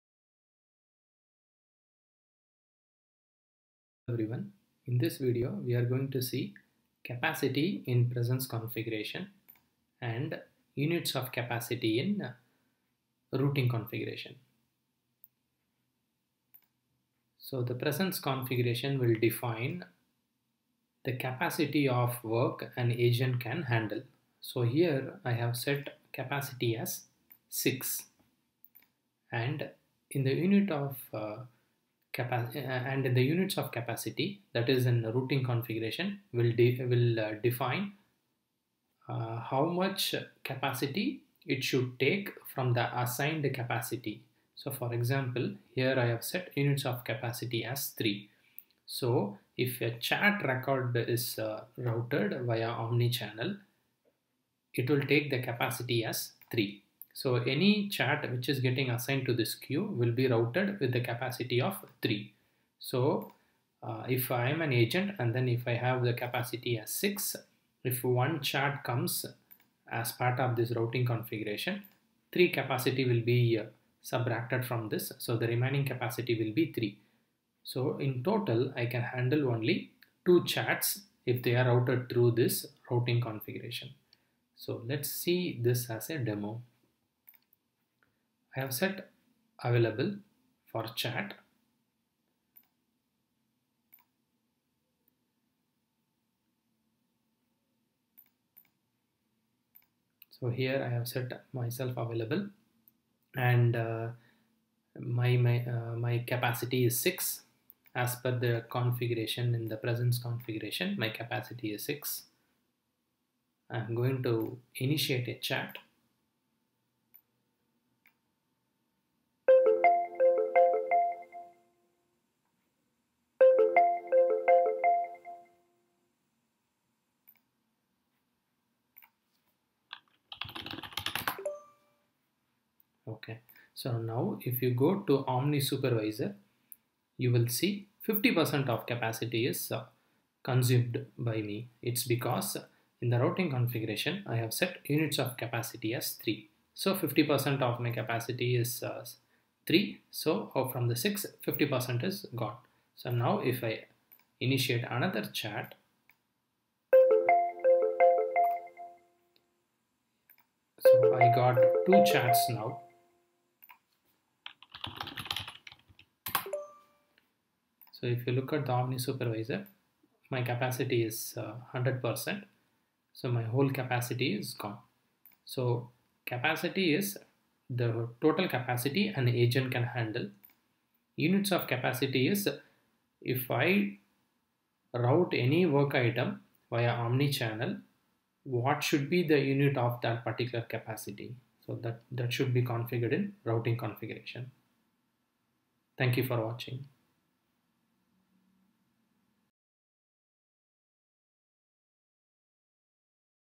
Units of Capacity in Routing Configurations and Capacity in Presence Configurations in Salesforce
It is important to understand Units of Capacity in Routing Configurations and Capacity in Presence Configurations Salesforce while setting up Omni-Channel.